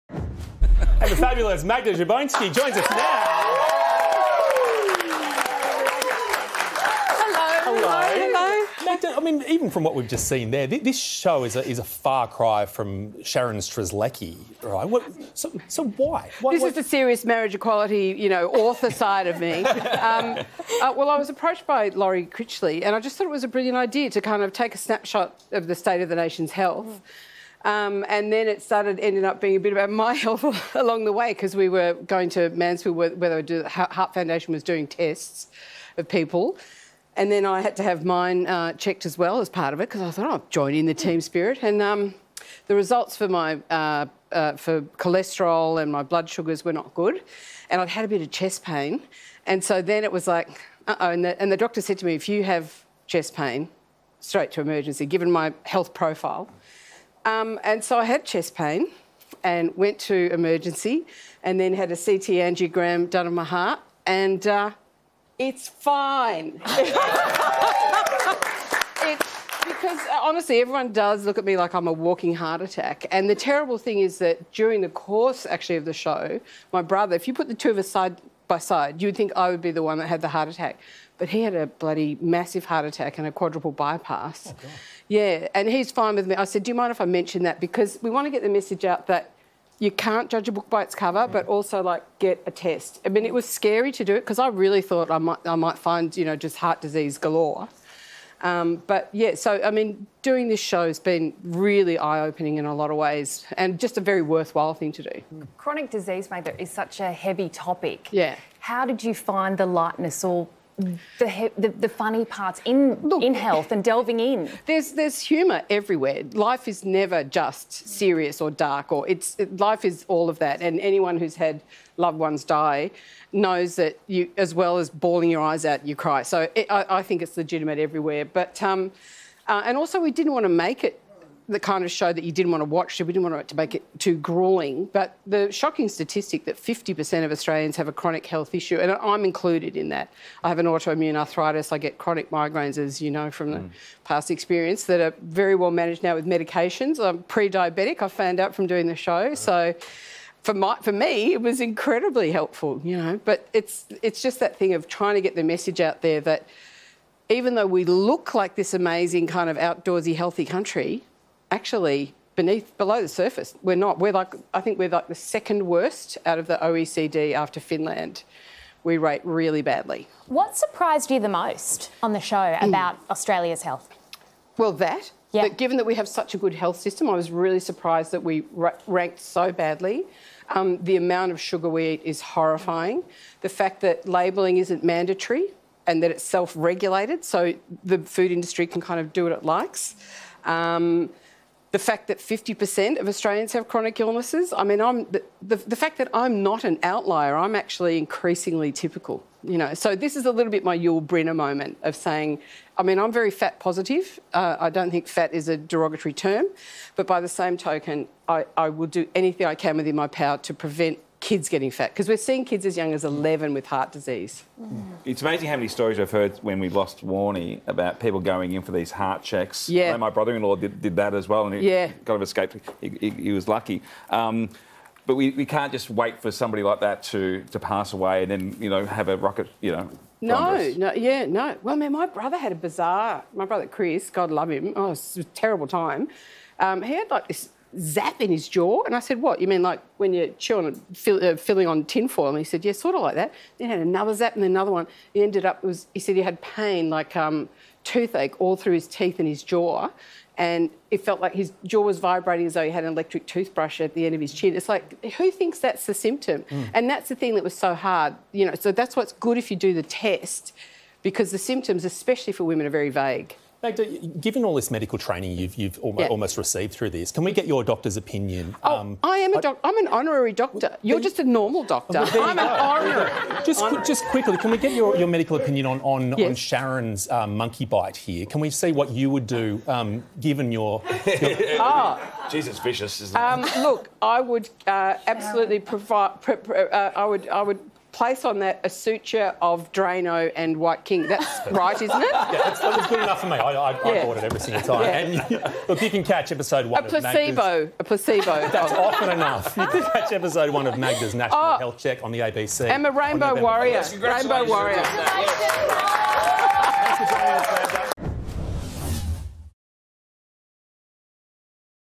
Magda Szubanski: Magda Szubanski On The Health Test That Changed Everything
While Magda Szubanski was filming her latest show about the state of Australians' health, the results of her own tests changed the course of the show.